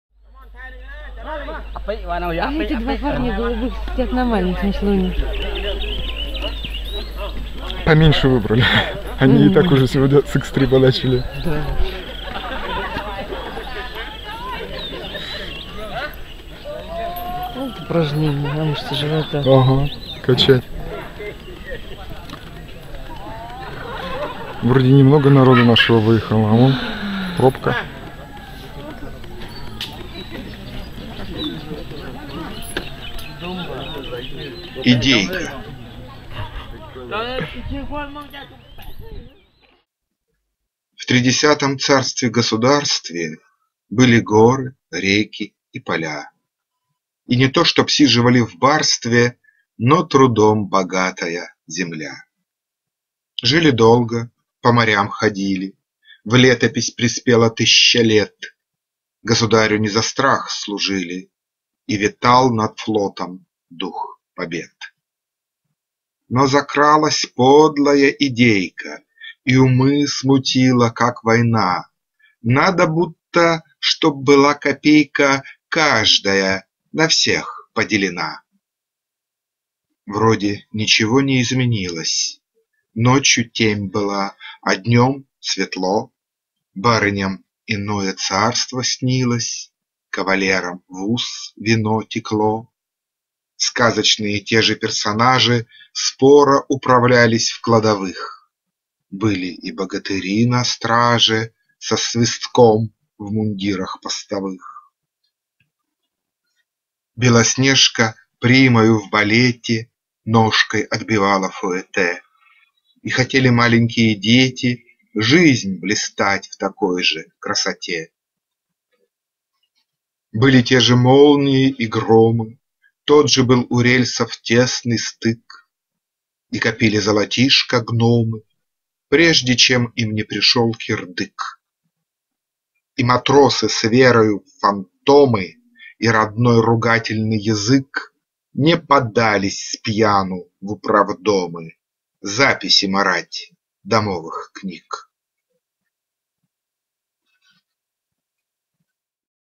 Идейка
Олег Кустов читает стихотворение "Идейка".

Стихотворение 2015 года из сборника "Большие каникулы" (2018).

В тридесятом царстве-государстве
Были горы, реки и поля,
И не то, чтоб сиживали в барстве,
Но трудом богатая земля.

Жили долго, по морям ходили,
В летопись приспела тыща лет.
Государю не за страх служили,
И витал над флотом дух побед.

Но закралась подлая идейка
И умы смутила, как война:
Надо будто, чтоб была копейка
Каждая на всех поделена.

Вроде ничего не изменилось:
Ночью темь была, а днём – светло,
Барыням иное царство снилось,
Кавалерам в ус вино текло.

Сказочные те же персонажи
Споро управлялись в кладовых,
Были и богатыри на страже
Со свистком в мундирах постовых.

Белоснежка примою в балете
Ножкой отбивала фуэте,
И хотели маленькие дети
Век блистать в такой же красоте.

Были те же молнии и громы,
Тот же был у рельсов тесный стык,
И копили золотишко гномы,
Прежде чем им не пришёл кирдык

И матросы с верою в фантомы
И родной ругательный язык
Не подались спьяну в управдомы
Записи марать домовых книг.